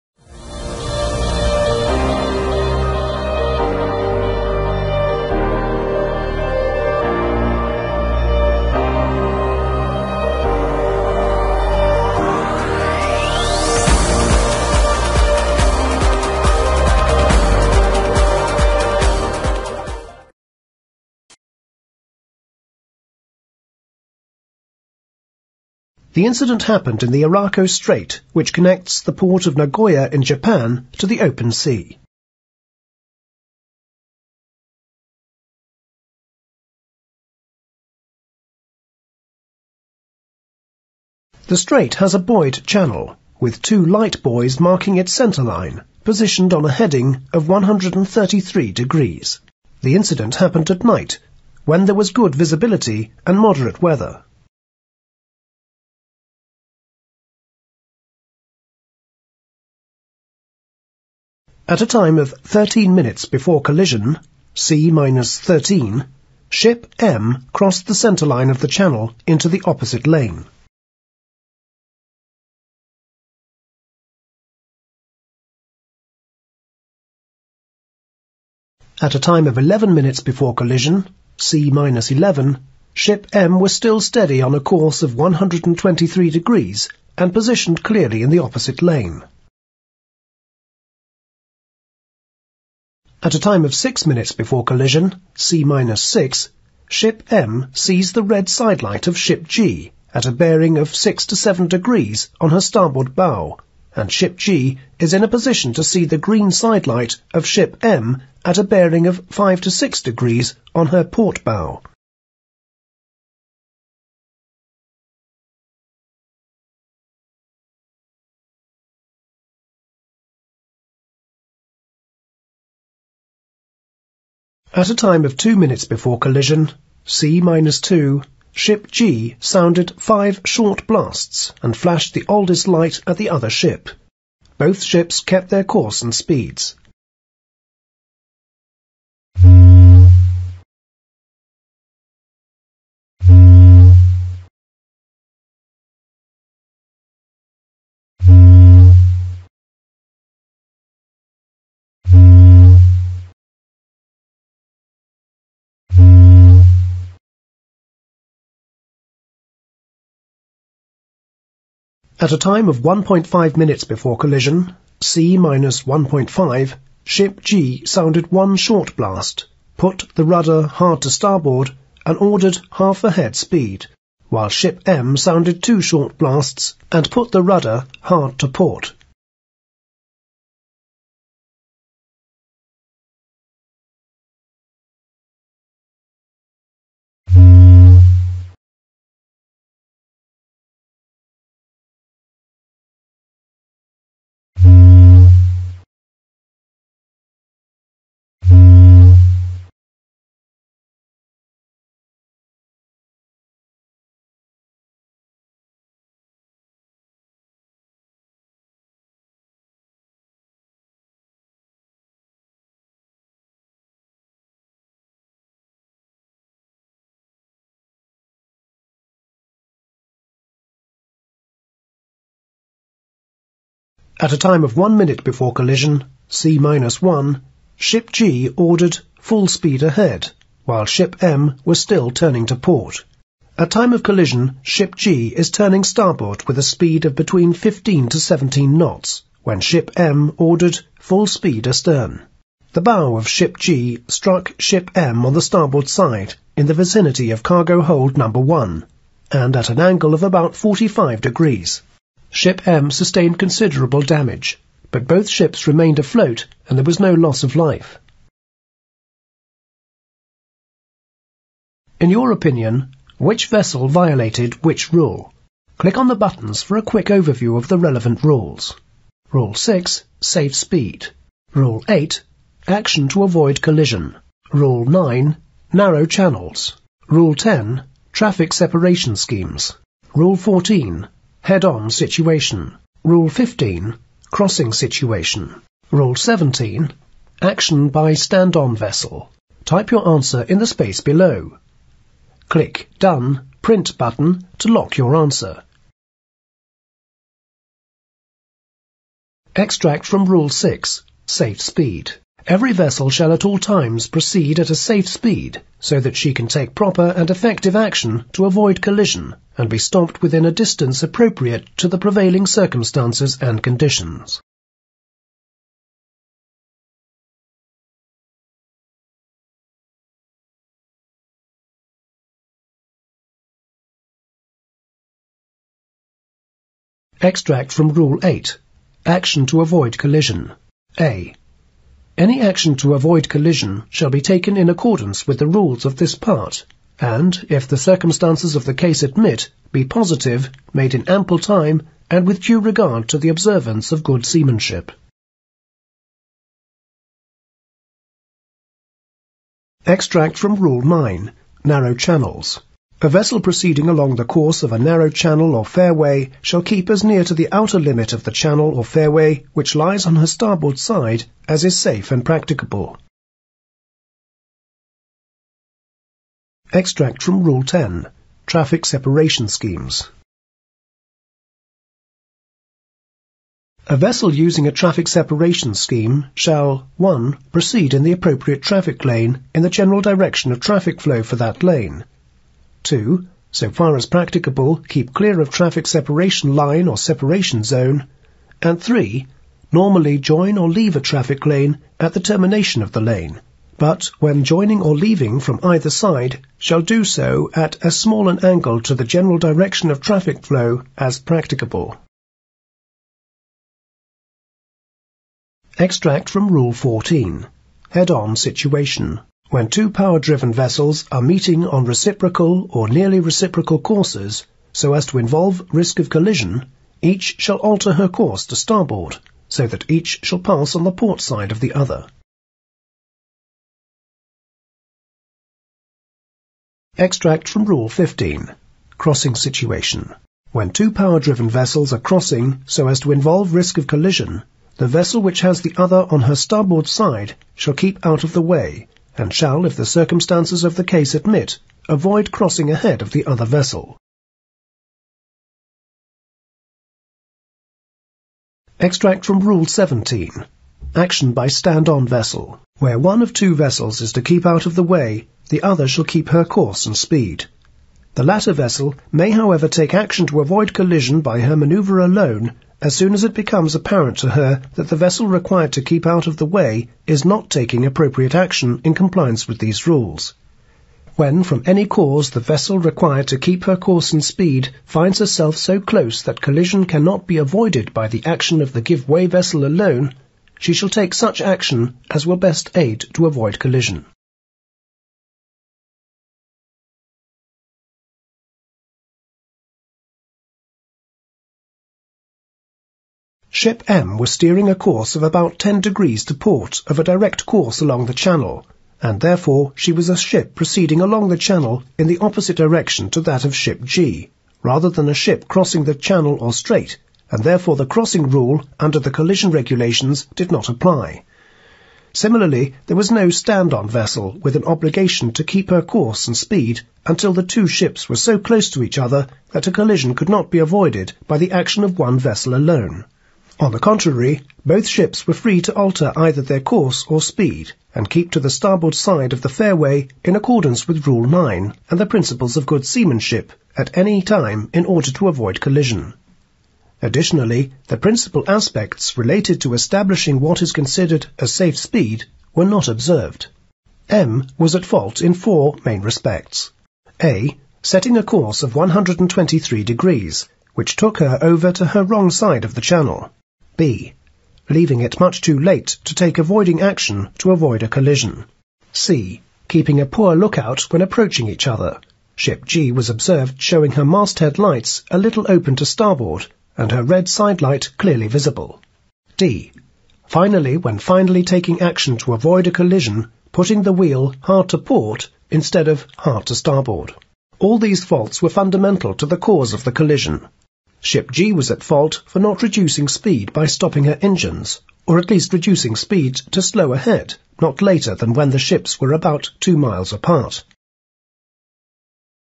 Vessel Collision Case Study 1
In this video will describe a real life maritime incident and explain the chain of events leading to a collision. it will highlight the rules of the Road being applied to the-situation and provide an opinion regarding the responsibility of each party involved an opinion regard the responsibility of each party involved in the collision.